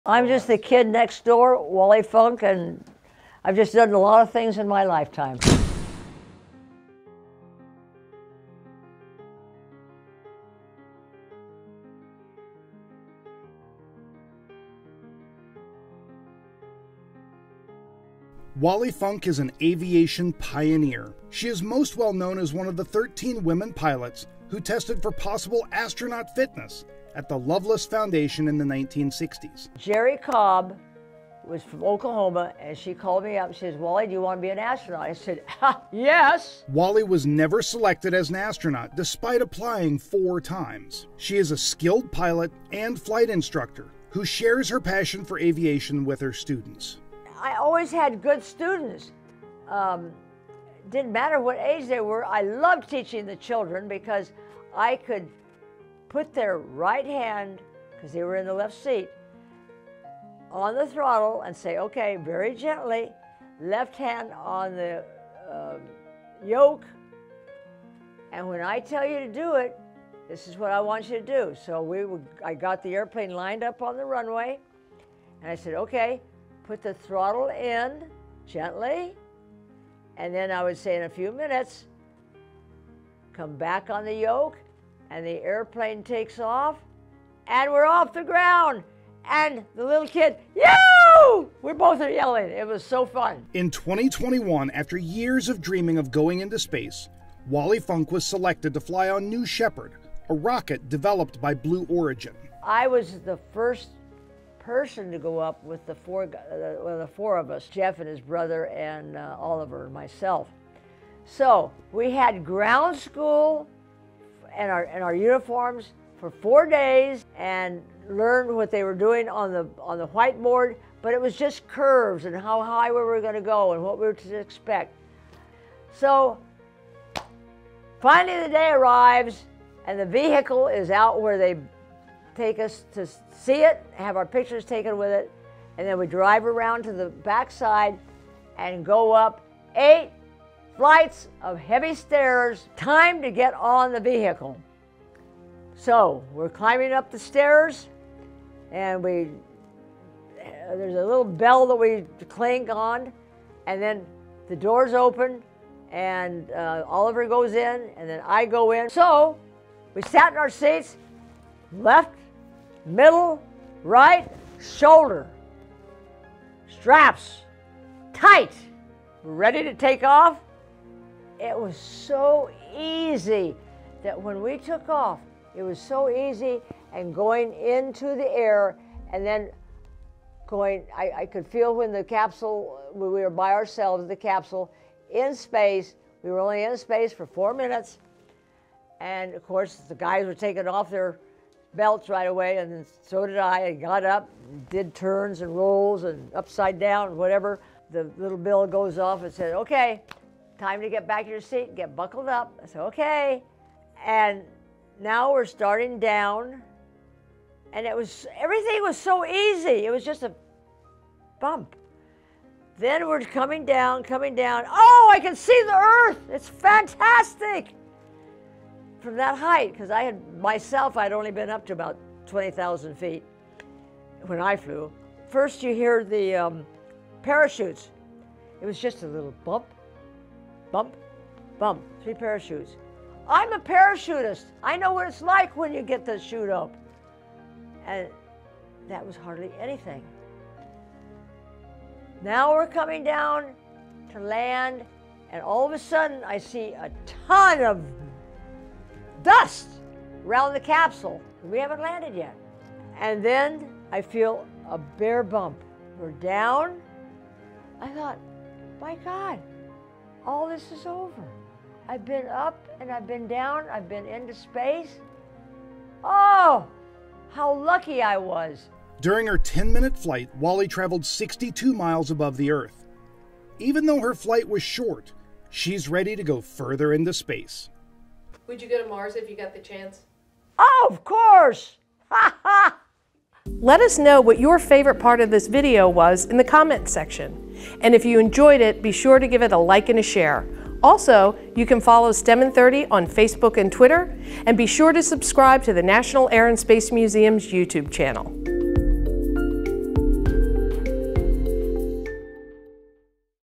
Wally Funk: Aviation Pioneer
Flying airplanes was something that Wally Funk seemingly always wanted to do. She had her first flying lesson when she was nine years old and by 20 had become a professional aviator. In 1961 she volunteered for the “Women in Space” program with the hopes of becoming an astronaut. Wally was one of the 13 women who, despite having passed all of the rigorous testing proving they were physically and mentally qualified for space travel, were denied the chance to become astronauts when the program was canceled. Funk continued to dream of going into space and in July of 2021 achieved her dream by flying onboard Blue Origin’s New Shepard rocket.